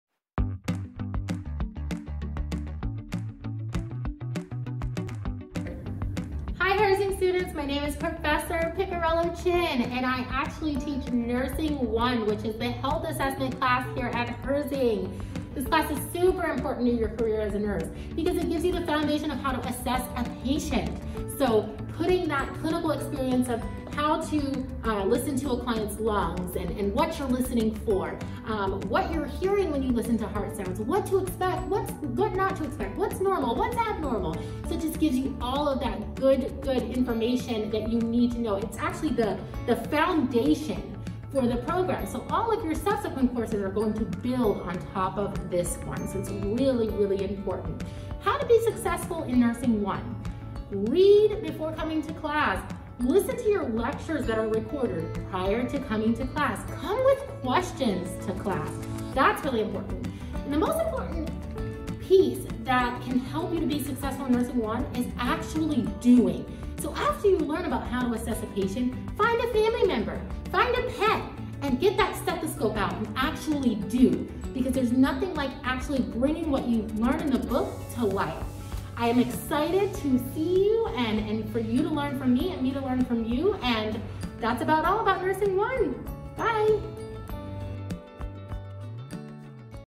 Class Overview | Professor Picariello-Chin in Orlando
We asked Professor Picarielle-Chin for a quick overview of her Nursing 1 class in Orlando, Florida. Check out what you can expect!